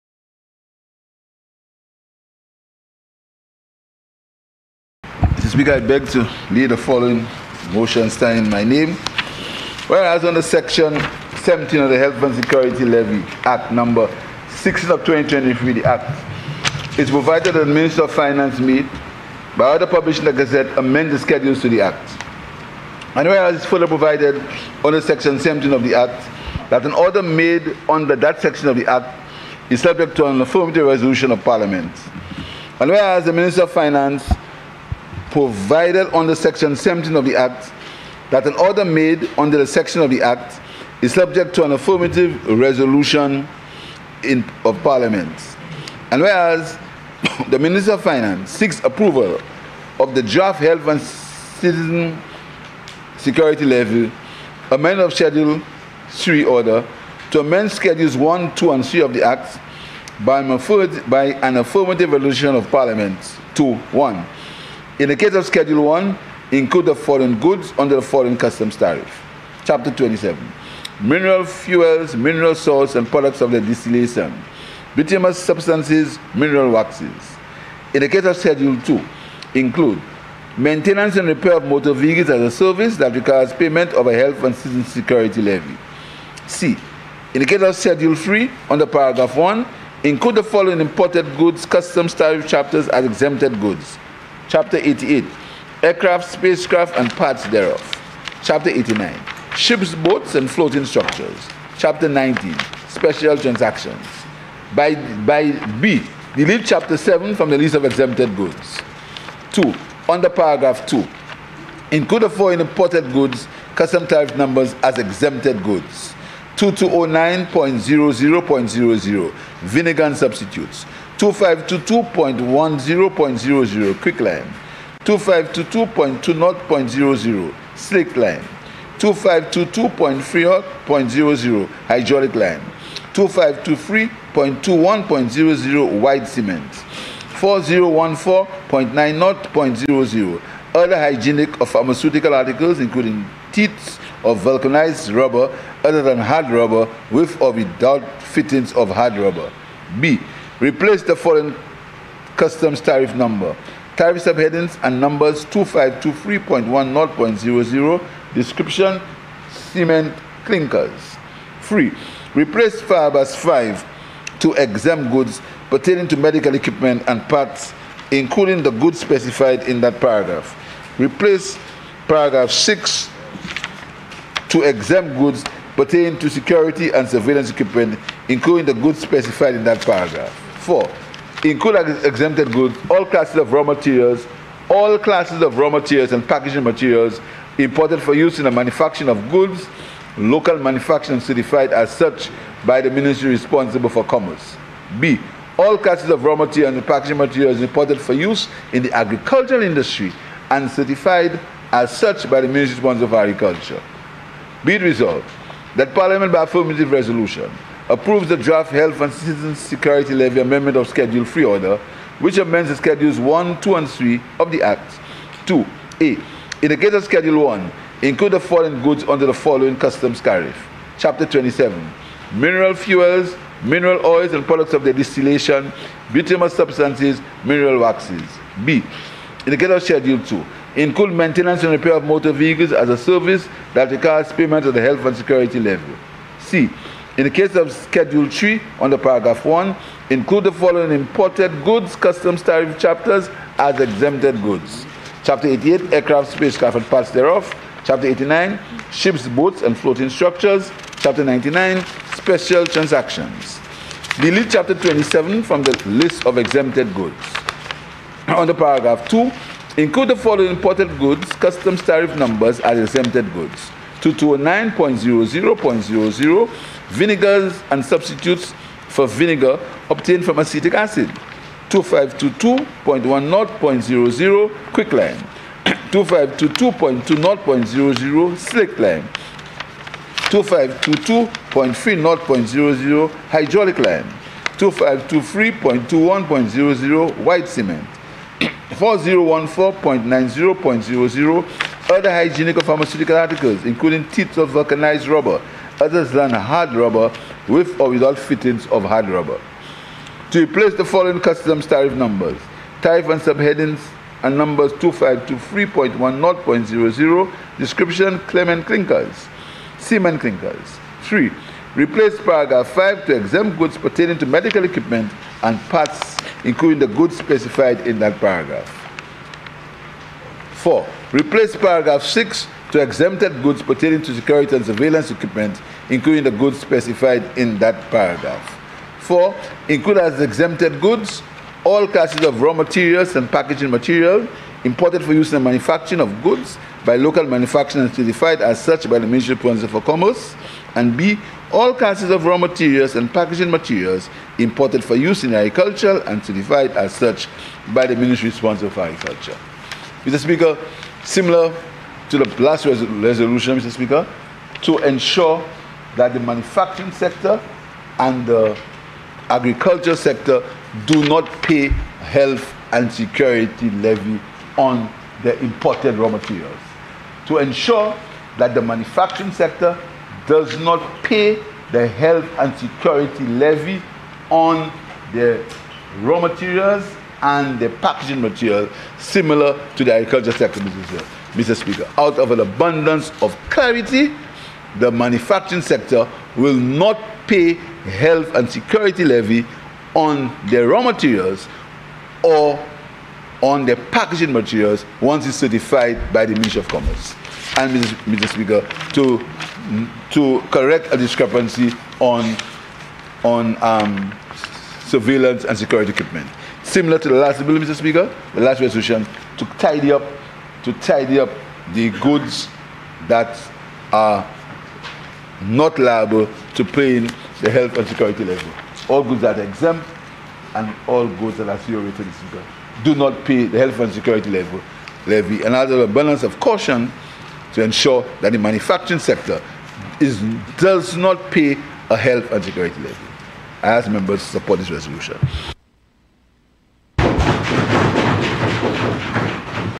Hon. Philip J. Pierre presents the Health and Citizen Security Levy (Amendment of Schedules 1,2&3)
Hon. Philip J. Pierre presents  the Health and Citizen Security Levy (Amendment of Schedules 1,2 and 3) Order